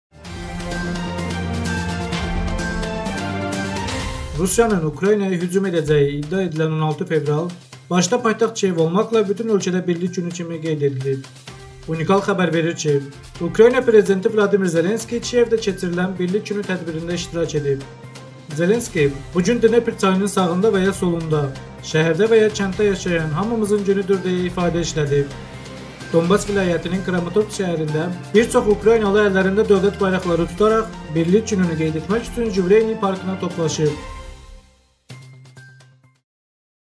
Ukraynada “Birlik Günü” Qeyd Olundu
Rusiyanın Ukraynaya hücum edəcəyi iddia edilən 16 fevral başda paytaxt Kiyev olmaqla bütün ölkədə “Birlik Günü” kimi qeyd edilib.

"Unikal" xəbər verir  ki, Ukrayna Prezidenti Vladimir Zelenski Kiyevdə keçirilən "Birlik Günü" tədbirində iştirak edib.

Zelenski "Bu gün Dnepr çayının sağında və ya solunda, şəhərdə və ya kənddə yaşayan hamımızın günüdür" ,- deyə ifadə  işlədib.

Donbass vilayətinin Kramatorsk şəhərində bir çox ukraynalı əllərində dövlət bayraqları tutaraq "Birlik Günü"nü qeyd etmək üçün Juvileyniy parkına toplaşıb.